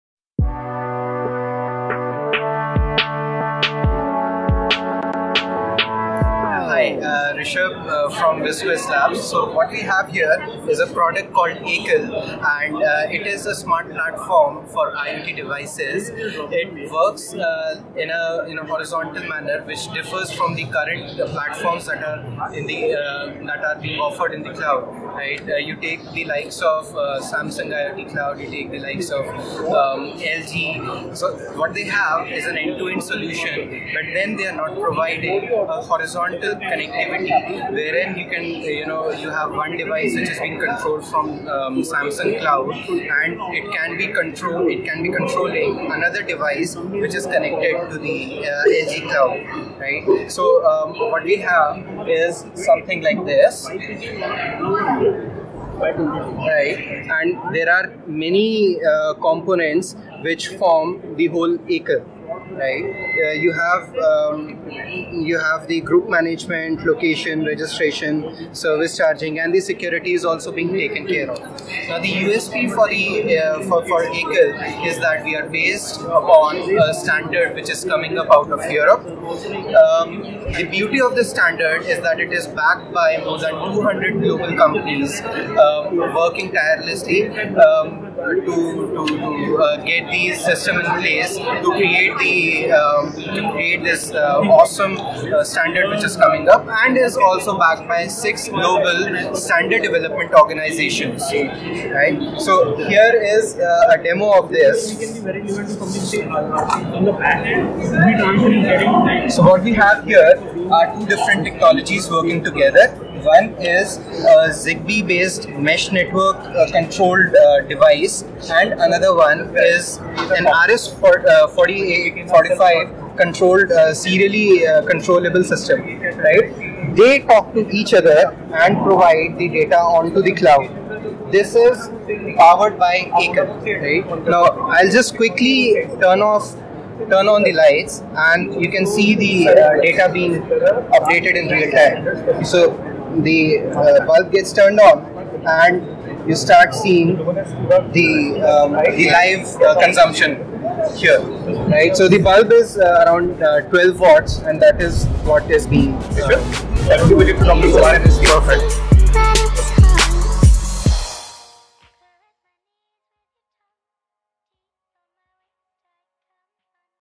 Startup Showcase: Wizquest
With Ekal, you can build truly scalable IoT solutions. You don't have to invest in creating a cloud platform for your solution.